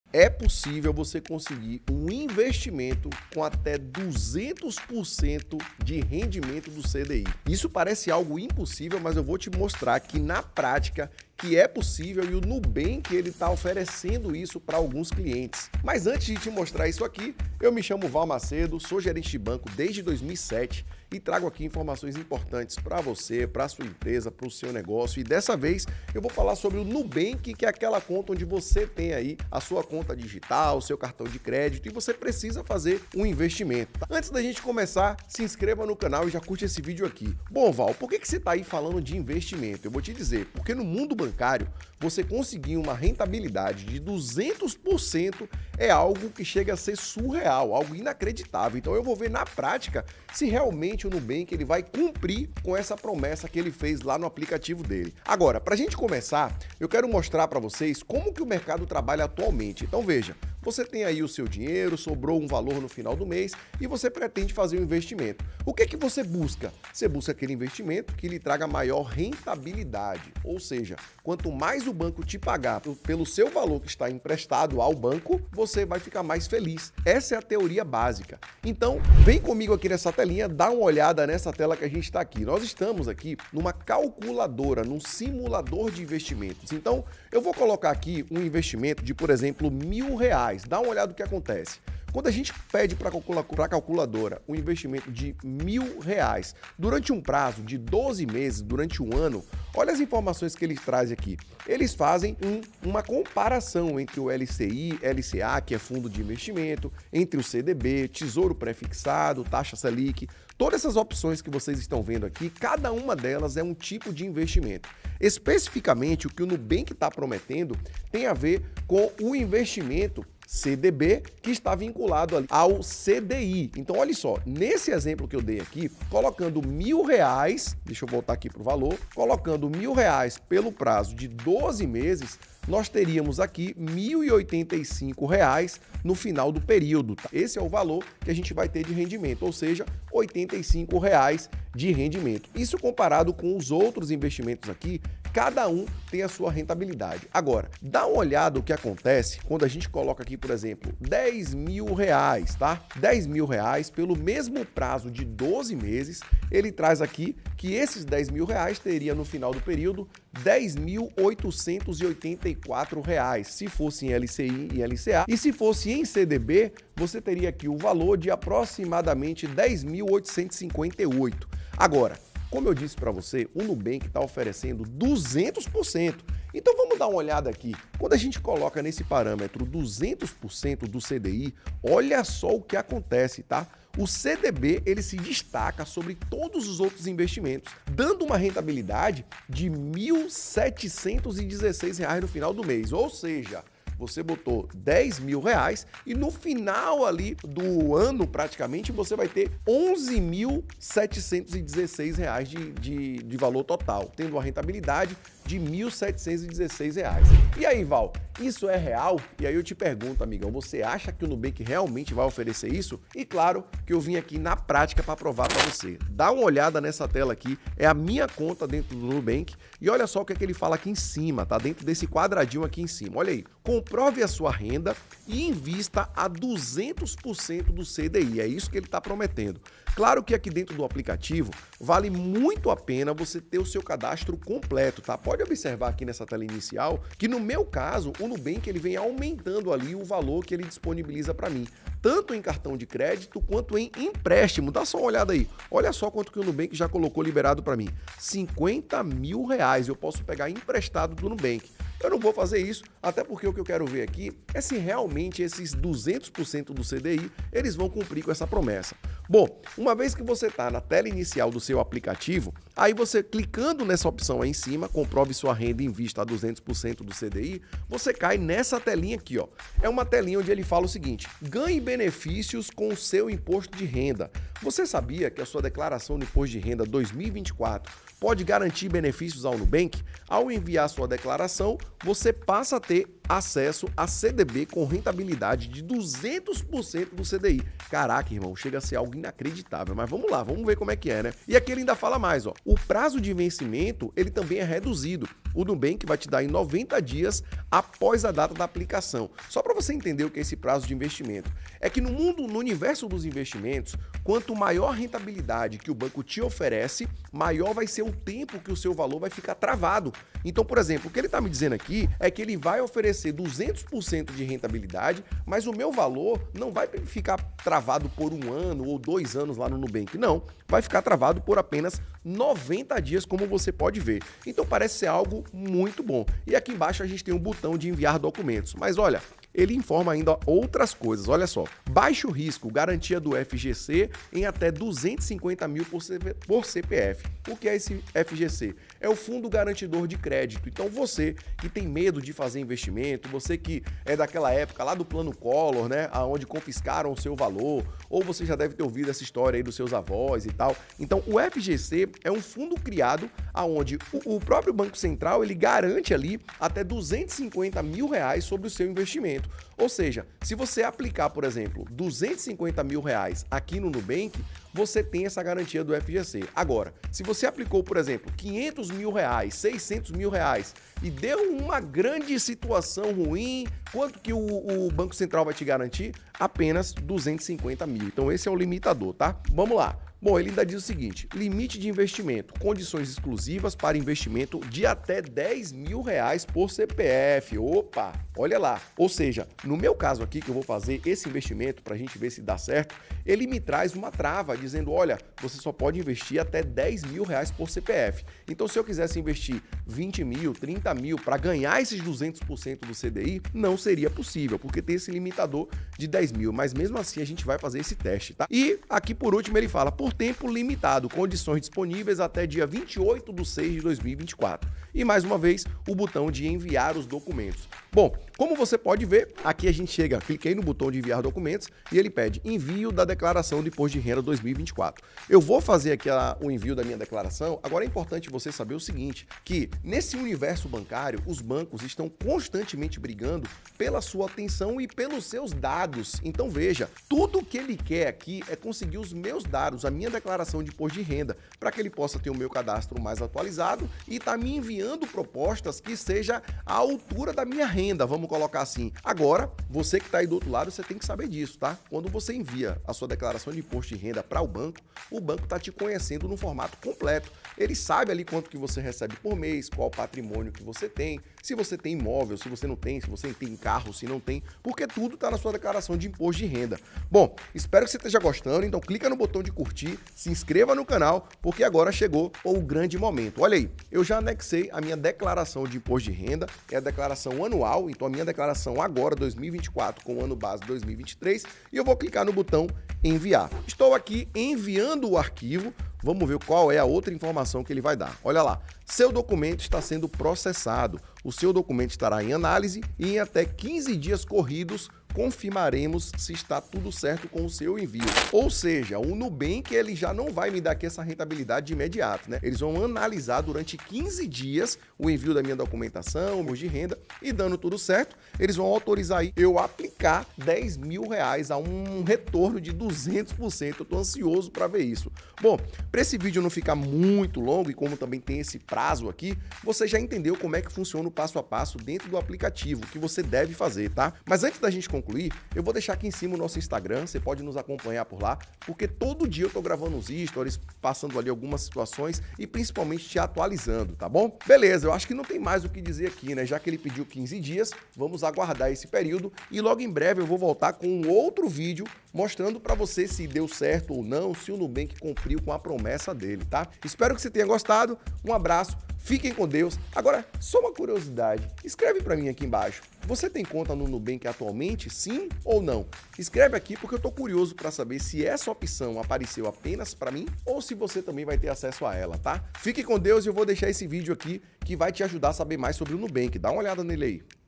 NUBANK: Como investir dinheiro e conseguir 200% do CDI. Na prática!!!
✅  Para empresa LTDA, ME e grande empresas: Descubra os Segredos da conta PJ:

Quanto rende colocar um dinheiro na caixinha nubank. Essa é a duvida de muitas pessoas e por isso resolvi fazer esse vídeo parta te mostrar na prática quanto rende se você colocar grana nesse investimento nubank.

A promessa do nubank é de 200% do CDI e por isso fiz a aplicação de 10 mil nesse investimento para ver se realmente o nubank vai entregar o que promete. Veja o vídeo para ver todos os detalhes.

Não esqueca que o melhor investimento do mundo é a diversificação. Sem ela, você estará colocando todos os ovos em um único local. Por isso sempre que pensar em investimento, analise qual vai ser o retorno e quantos % você terá de rentabilidade.

O investimento nubank tem atraído muitas pessoas que buscam rentabilidade em suas econômicas, contudo vale lembrar que fazer um investimento é pensar no longo prazo. por esse motivo fiz questão de trazer detalhes para que você consiga observar o retorno que vai ter.

Saber qual o melhor investimento para você também passa pelo seu perfil de investidor, que é uma forma de comprovar o quanto você está disposto a expor ao risco e retorno.

Aplica as dicas e antes de fazer o investimento na caixinha do nubank, veja todas as opções de investimento.

Abração,

Wal Macedo

00:34 Nubank
01:27 Simulador investimento nubank
03:01 Investimento de 200%
04:41 Imposto de renda para nubank
05:54 Fundo Garantidor de Crédito - FGC
07:37 Enviando informações para a caixinha nubank
08:28 Análise do nubank